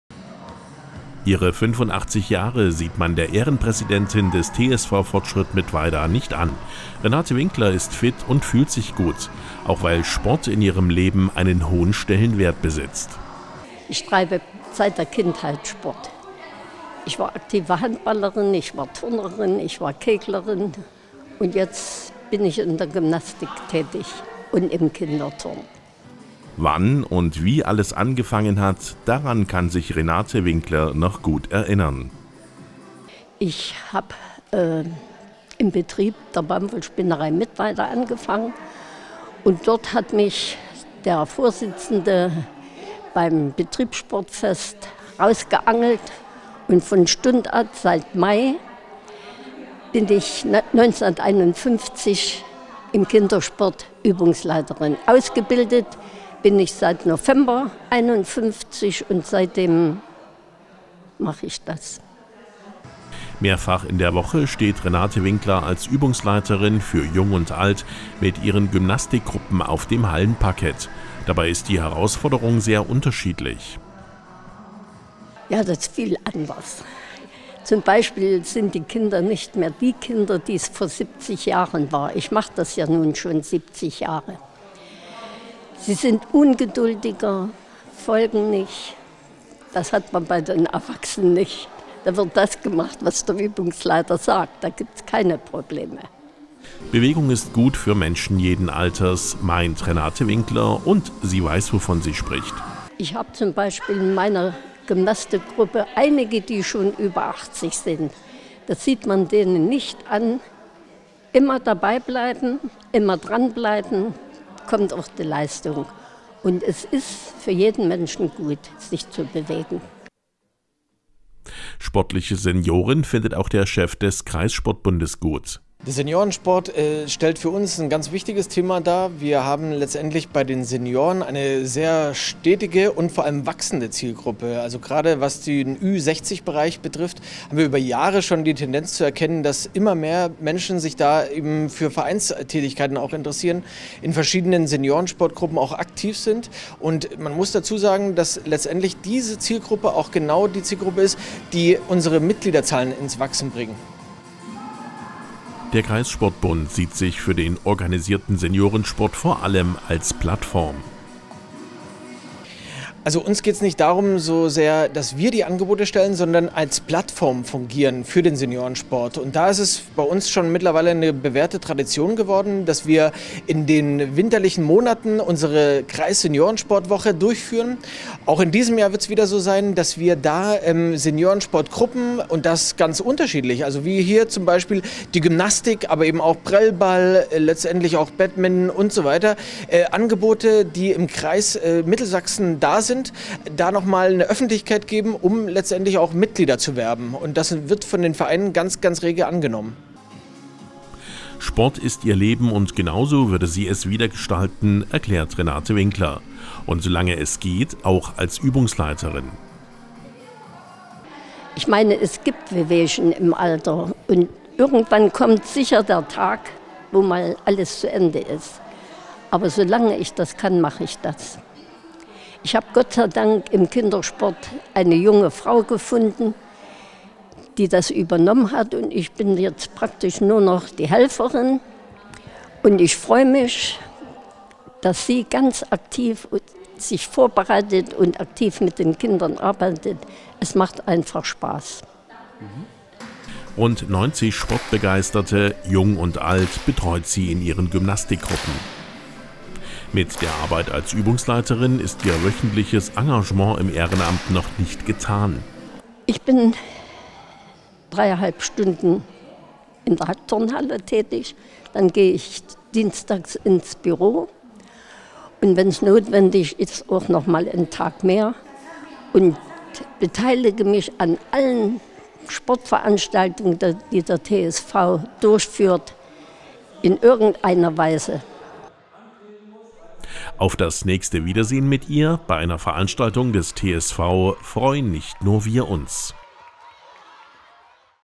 Ein Leben für den Sport
In diesem Beitrag berichten wir über eine Frau, die seit mehr als 70 Jahren als Übungsleiterin tätig ist. Dazu besuchten wir die Gymnastikgruppe des TSV „Fortschritt“ Mittweida.